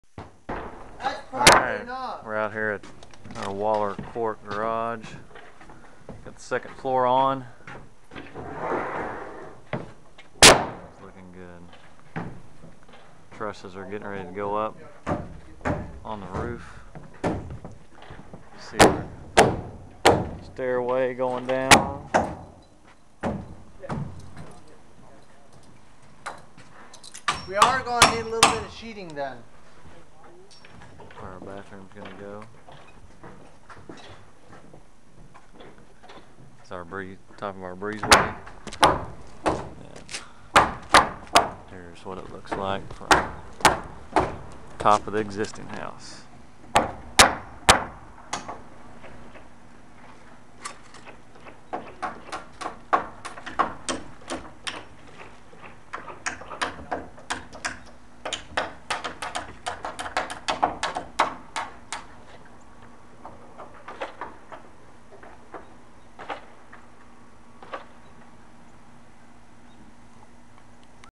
Waller, Garage Addition
Framing the 2nd floor of the garage